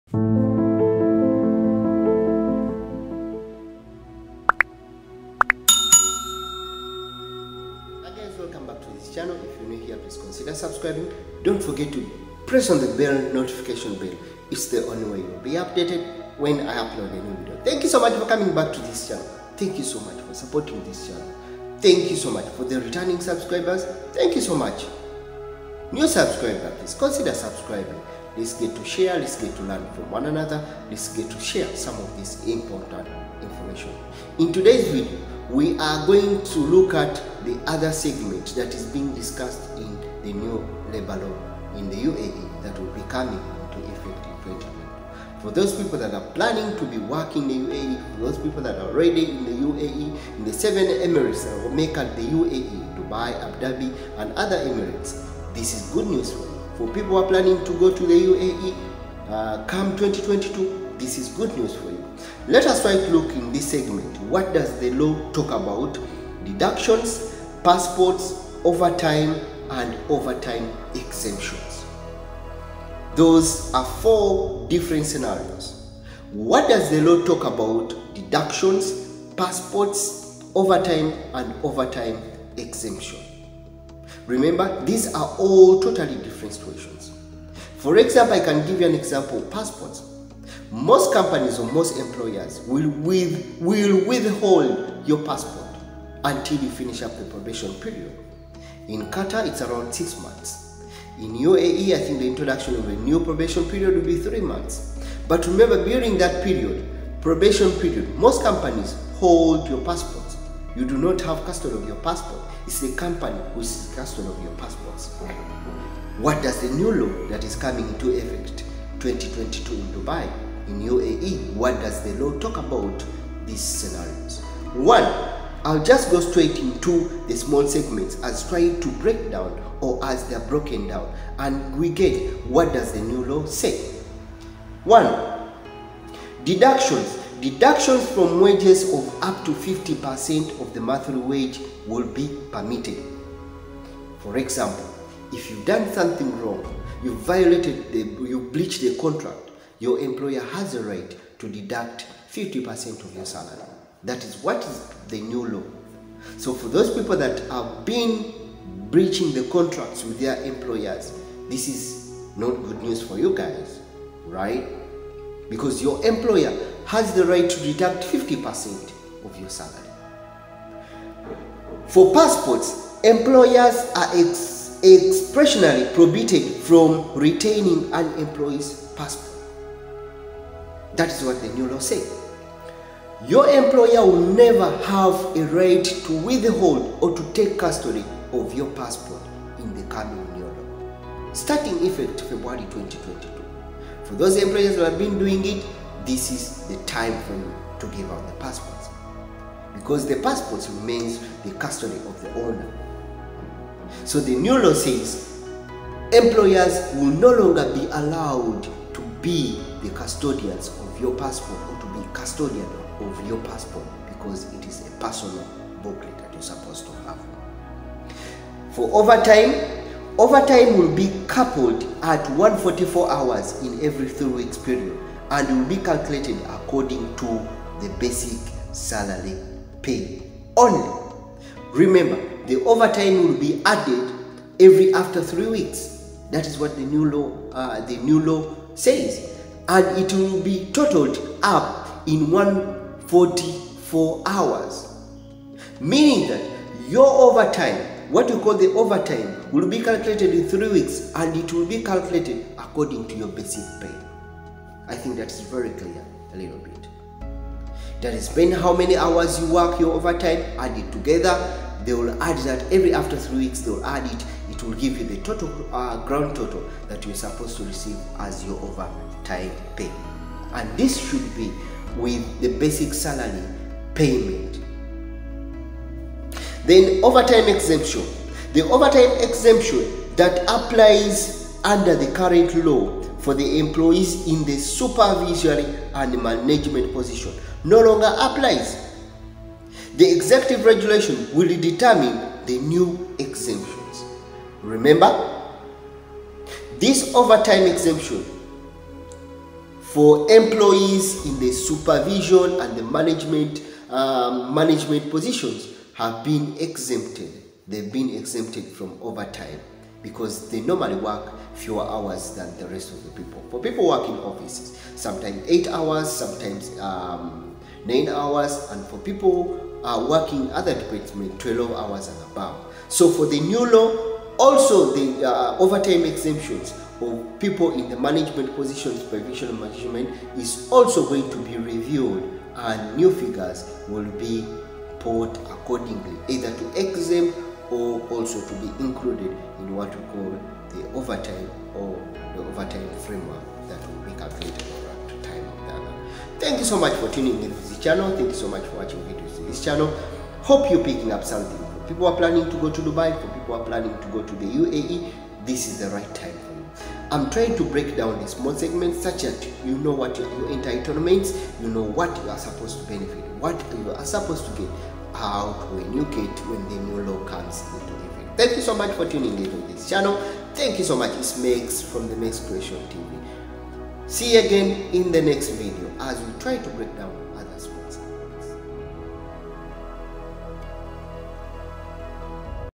The New UAE Labour Law 2022 No More Salary Deductions, Overtime Pay Increased 2k subscribers
The New UAE Labour Law 2022 ,No More Salary Deductions, Overtime Pay Increased and Will be Calculated According to the Basic Salary | Mexcreationtv